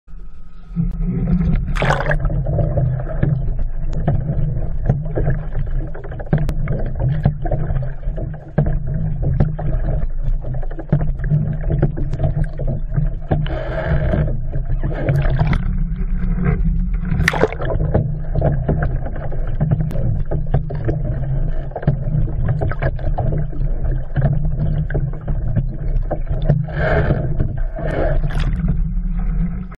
paddeling upside down
paddeling a kayak upside down in warm waters